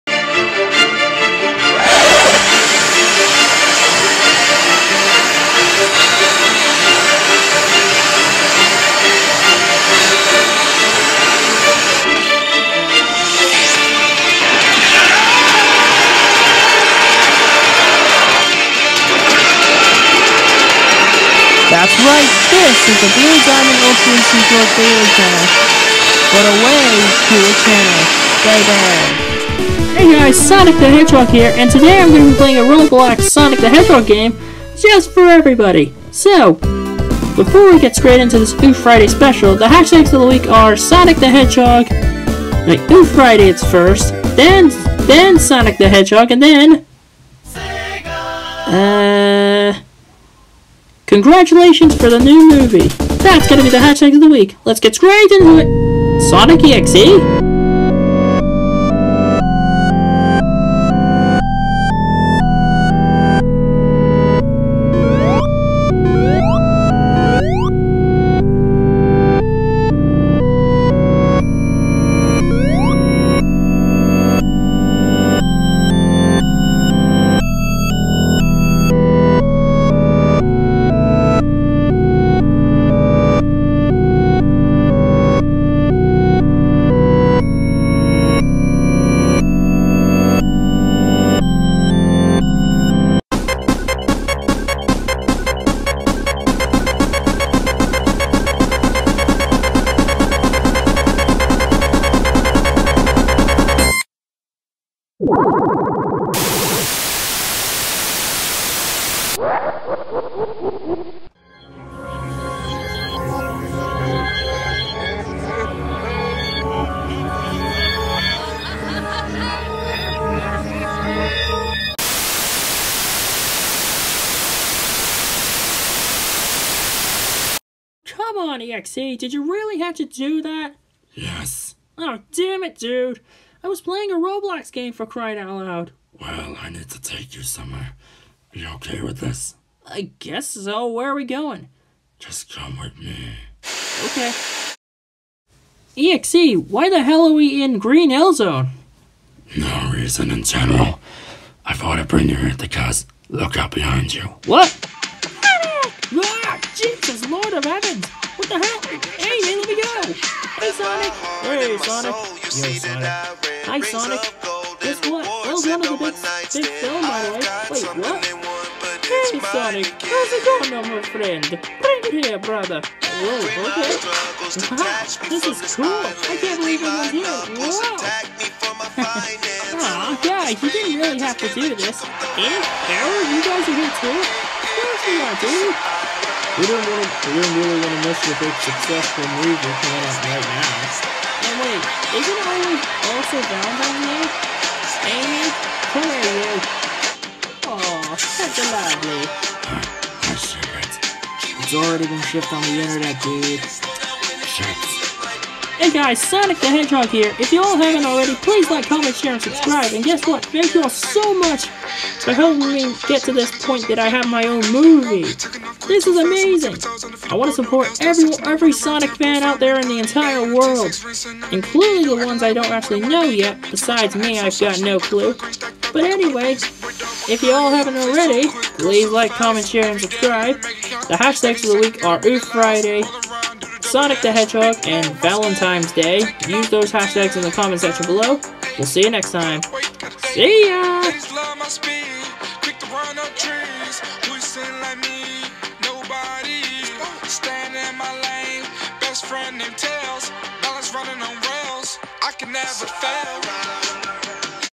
Oofirday sonic the hedgehog
Sonic the hedgehog has become a big success for all around the world know and then I will be watching it very soon I will not spoil it for anyone out there who is not watched it yet the hashtags of the week are down here